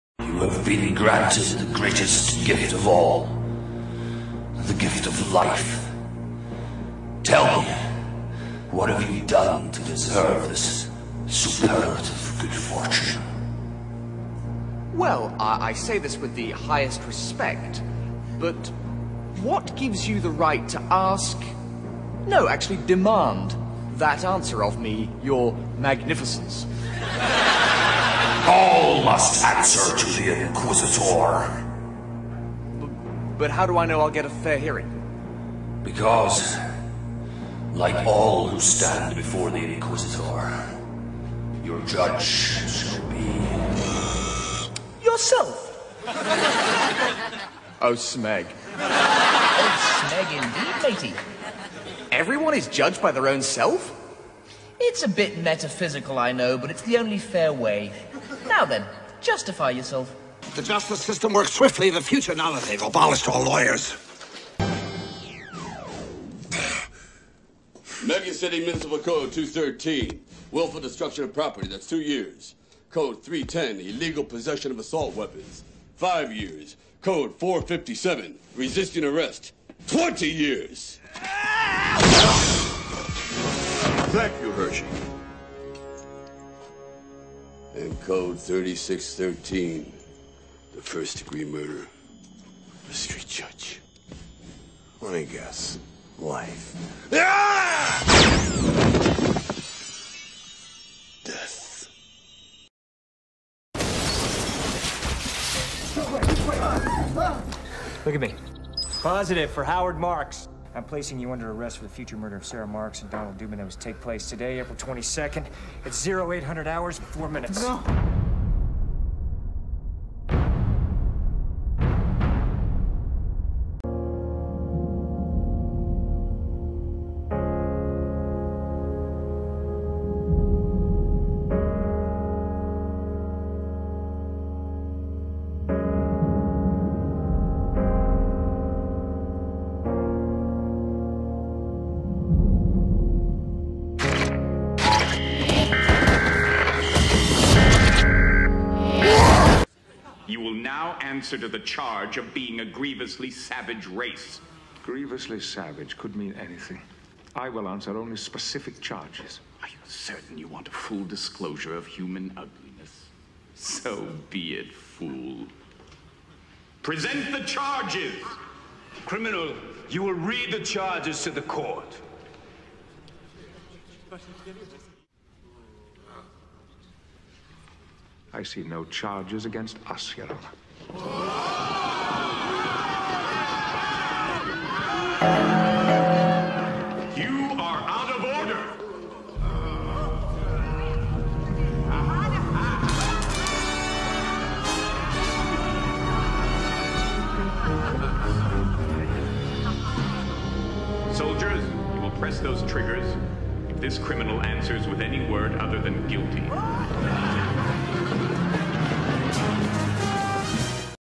The Future of the Law
I put this little montage together for a presentation that my pupilage team did for the Wesley E. Brown Inn of Court.  Honestly, there are probably a lot of better clips out there to use, but this was just anecdotal in nature to set up our presentation.